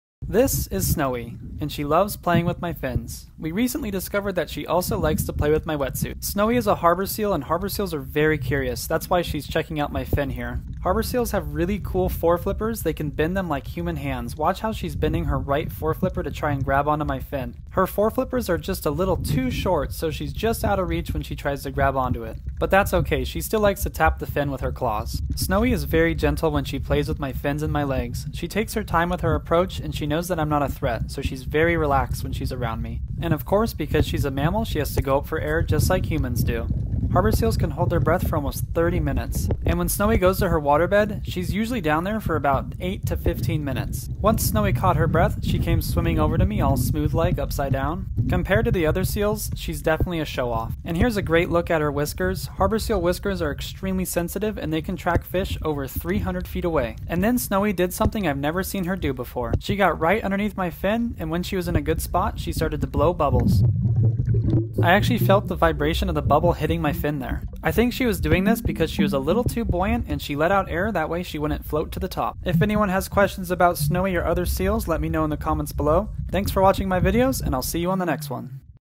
This Seal Blew Bubbles On My Fin
Snowy was letting out air to achieve neutral buoyancy

**if a seal comes up to you never touch them or try to pet them. Seals are protected under the Marine Mammal Protection Act. Seals can and will bite. The encounter is on their terms.